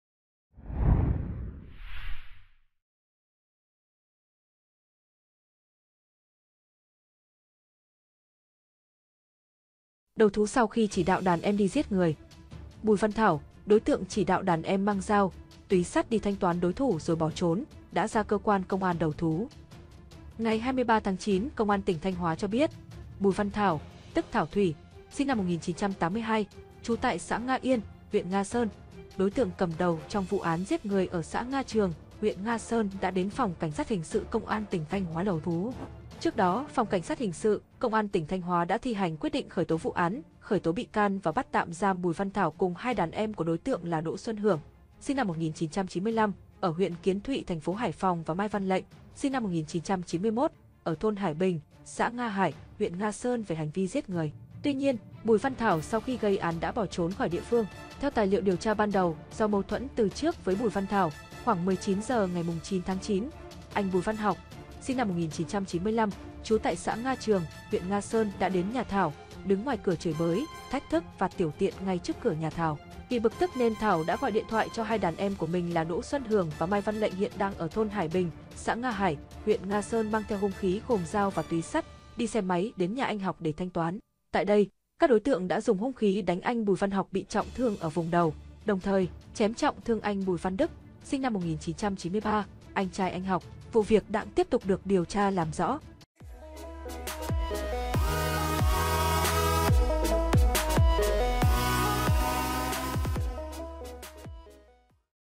Đầu thú sau khi chỉ đạo đàn em đi giết người
Bùi Văn Thảo - đối tượng chỉ đạo đàn em mang dao, túyp sắt đi "thanh toán" đối thủ rồi bỏ trốn - đã ra cơ quan công an đầu thú.
Ngày 23/9, Công an tỉnh Thanh Hóa cho biết, Bùi Văn Thảo (tức Thảo Thủy, SN 1982, trú tại xã Nga Yên, huyện Nga Sơn) - đối tượng cầm đầu trong vụ án giết người ở xã Nga Trường, huyện Nga Sơn đã đến Phòng Cảnh sát hình sự Công an tỉnh Thanh Hóa đầu thú.

Trước đó, Phòng Cảnh sát hình sự, Công an tỉnh Thanh Hóa đã thi hành Quyết định khởi tố vụ án, khởi tố bị can và bắt tạm giam Bùi Văn Thảo cùng 2 đàn em của đối tượng là Đỗ Xuân Hưởng (SN 1995, ở huyện Kiến Thụy, thành phố Hải Phòng) và Mai Văn Lệnh (SN 1991, ở thôn Hải Bình, xã Nga Hải, huyện Nga Sơn) về hành vi giết người. Tuy nhiên, Bùi Văn Thảo sau khi gây án đã bỏ trốn khỏi địa phương.
Theo tài liệu điều tra ban đầu, do mâu thuẫn từ trước với Bùi Văn Thảo, khoảng 19h ngày 9/9, anh Bùi Văn Học (SN 1995, trú tại xã Nga Trường, huyện Nga Sơn) đã đến nhà Thảo, đứng ngoài cửa chửi bới, thách thức và tiểu tiện ngay trước cửa nhà Thảo.

Vì bực tức nên Thảo đã gọi điện thoại cho 2 đàn em của mình là Đỗ Xuân Hưởng và Mai Văn Lệnh hiện đang ở thôn Hải Bình (xã Nga Hải, huyện Nga Sơn) mang theo hung khí gồm dao và túyp sắt, đi xe máy đến nhà anh Học để "thanh toán".

Tại đây, các đối tượng đã dùng hung khí đánh anh Bùi Văn Học bị trọng thương ở vùng đầu. Đồng thời, chém trọng thương anh Bùi Văn Đức (SN 1993, anh trai anh Học).

Vụ việc đang tiếp tục được điều tra, làm rõ.